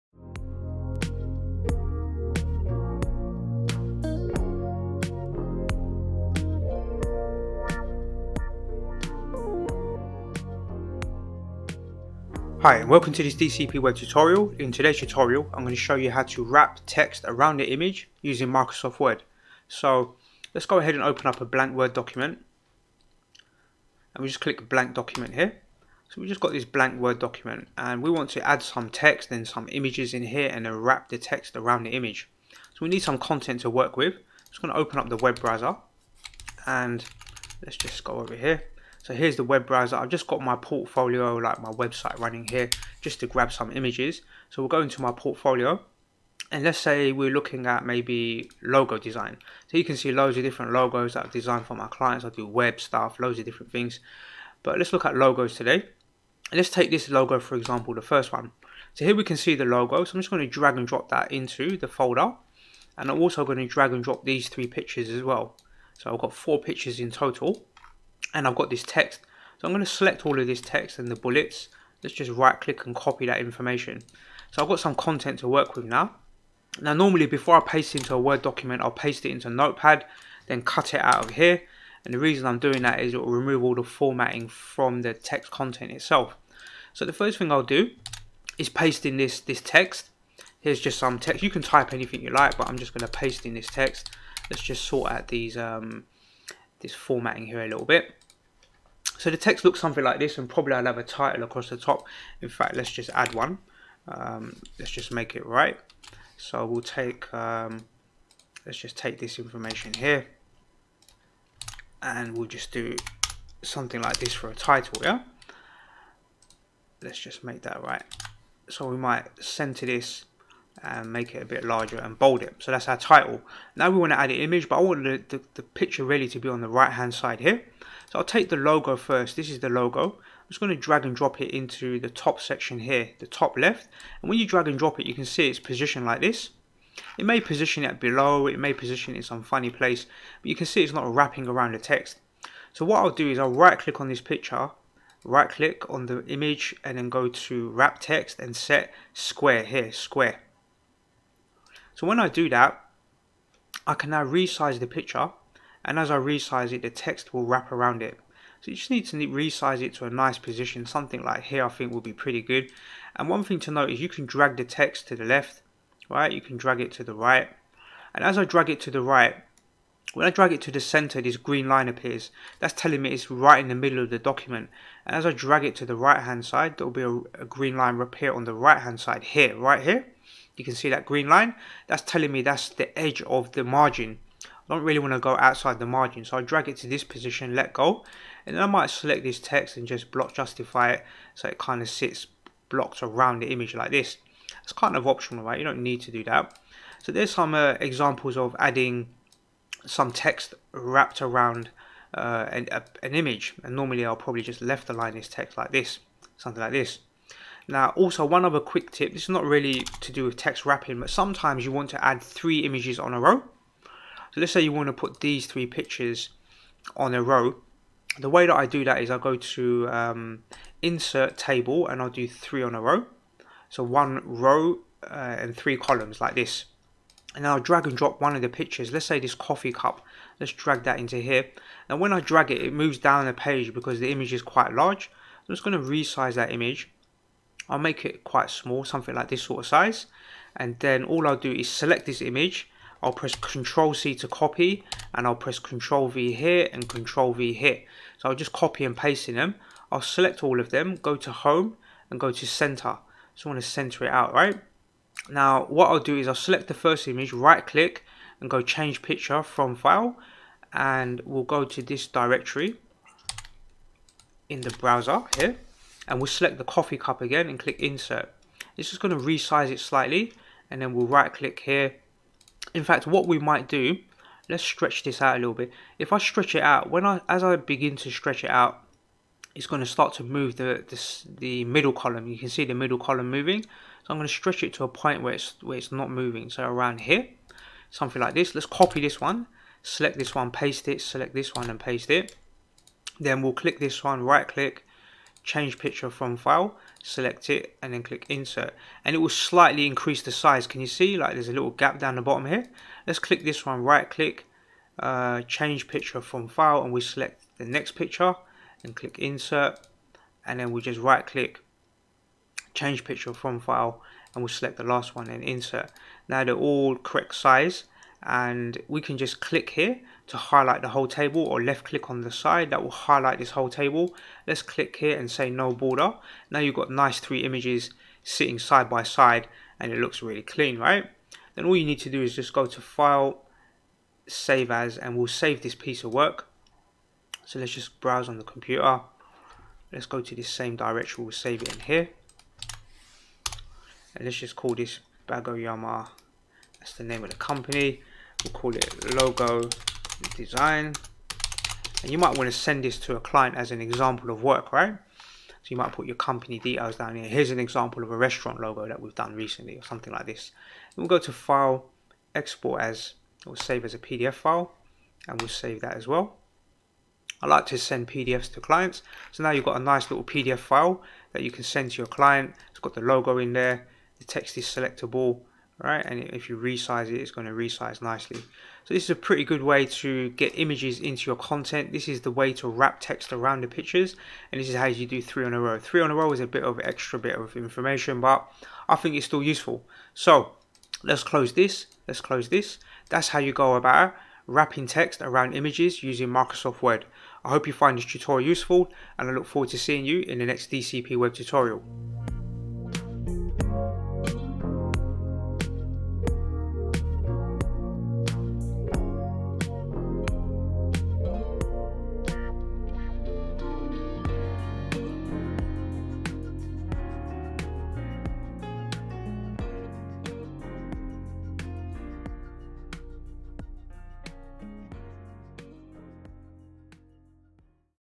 Wrap Text Around Pictures In Word
In this video tutorial, I will show you how to Wrap Text Around Pictures In Word.

Add a professional touch to your word documents by wrapping text around images for better presentation.

Need a WordPress website for your business? - Simply contact DCP Web Designers for a free quote today